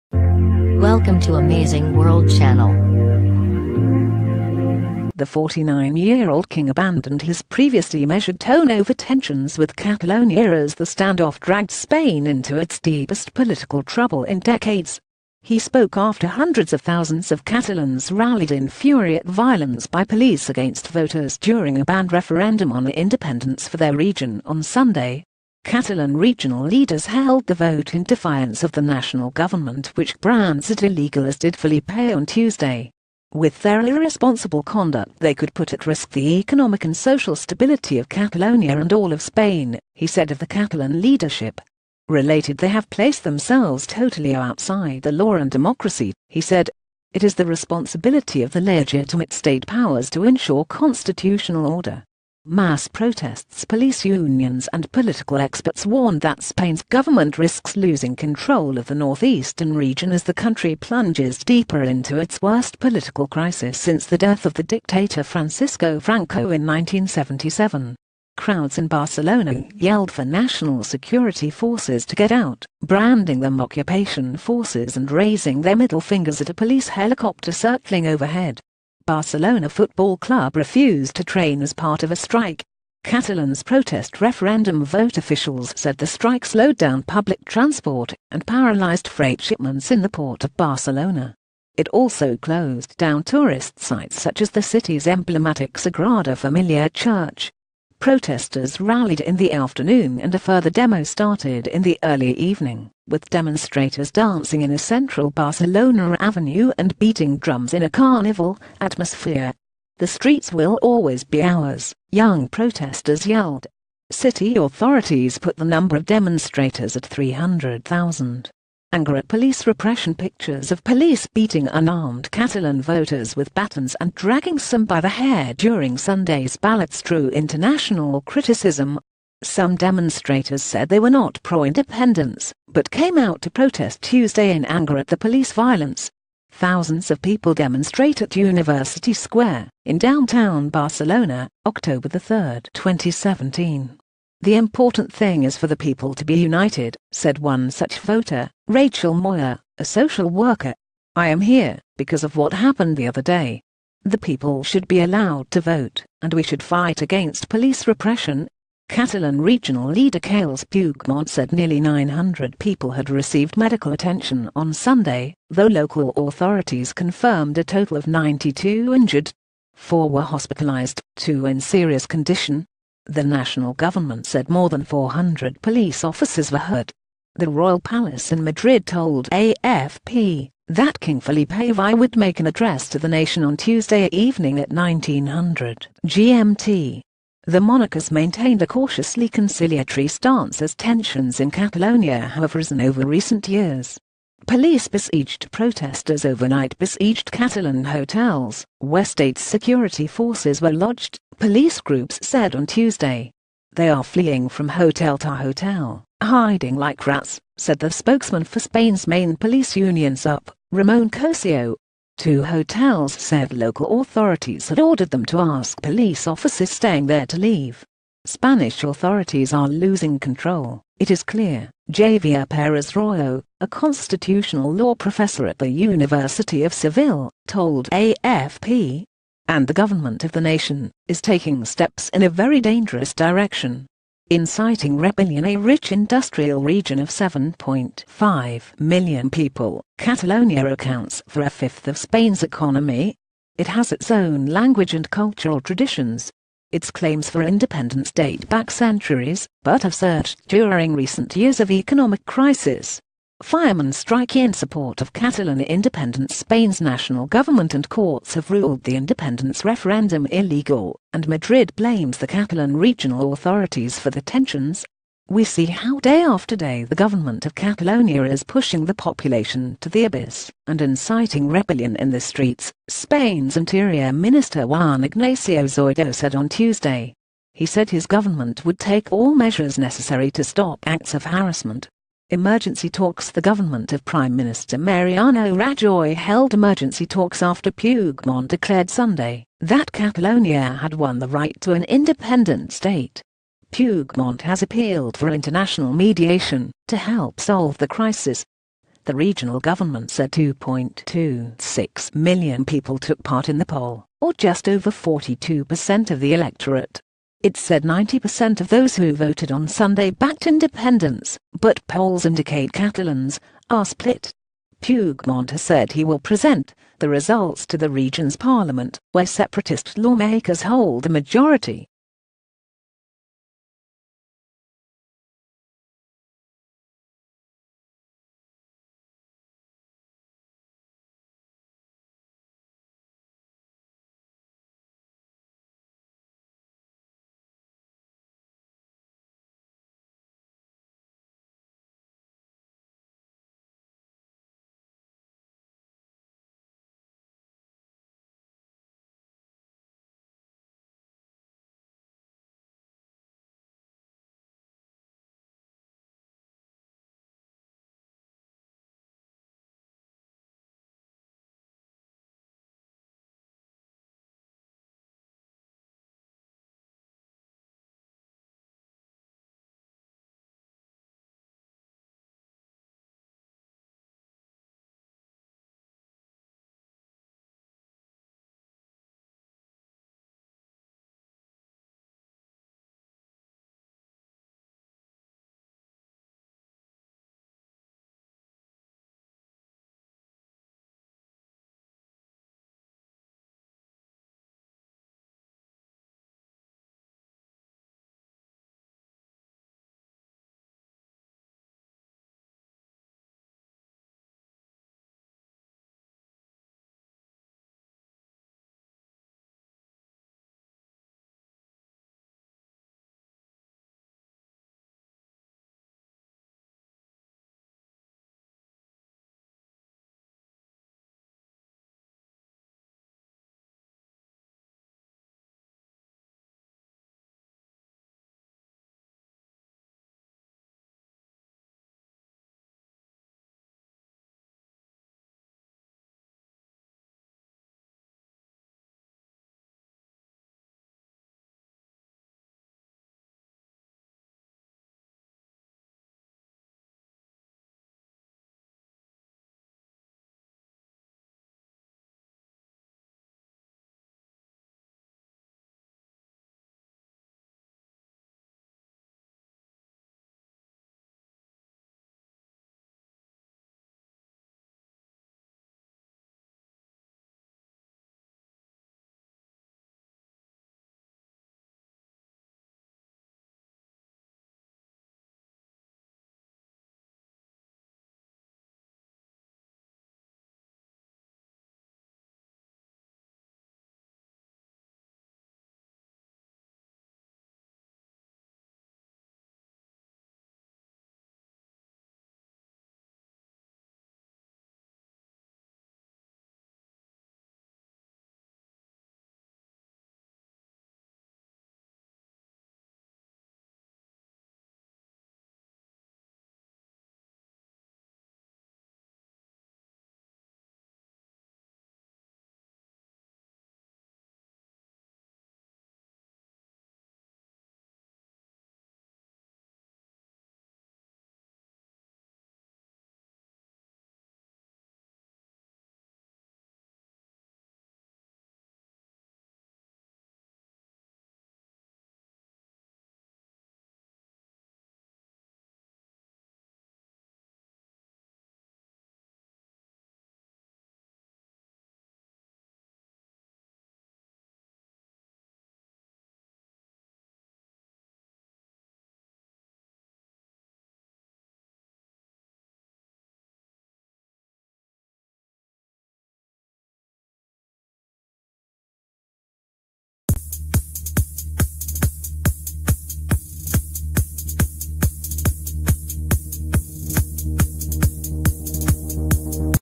'irresponsible conduct': spain's king denounces catalan independence bid News Today
'irresponsible conduct': spain's king denounces catalan independence bid
The 49yearold king abandoned his previously measured tone over tensions with Catalonia as the standoff dragged Spain into its deepest political trouble in decades. He spoke after hundreds of thousands of Catalans rallied in fury at violence by police against voters during a banned referendum on independence for their region on Sunday. Catalan regional leaders held the vote in defiance of the natio...